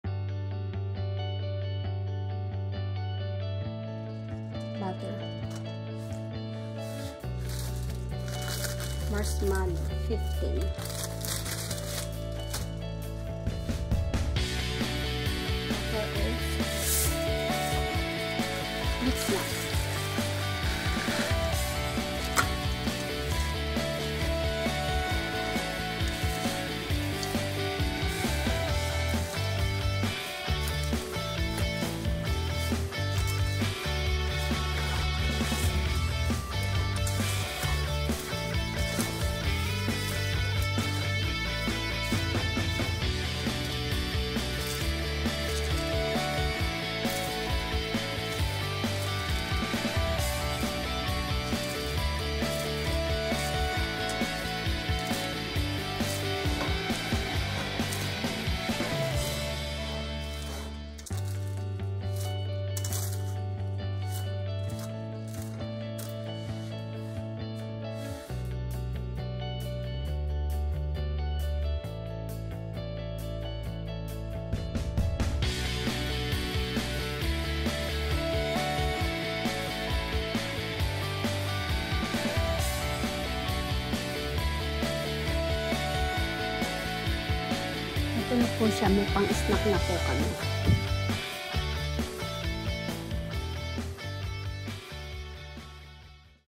SIMPLE/ MASARAP/DI MASYADONG MATAMIS "SNACK BAR"😍❤️👍
Try my delicious snack ( not too sweet)
Butter, Marshmallow, Quaker Oats, Corn flakes and Mixed Nuts.
(You can use Honey for sweetener, or  any kind of nuts, or just rice crispy, or corn flakes, you can put chocolate, or  put  more marshmallow  if you want a little bit more sweet.